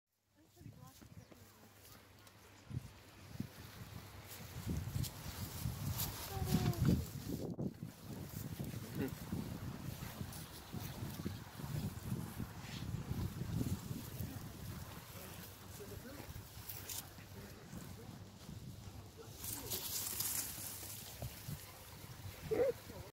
friendly Deer doesn't like to be touched
A friendly and cute deer in the park. But I failed to pet him. Sorry, deer. I didn't want to scare you...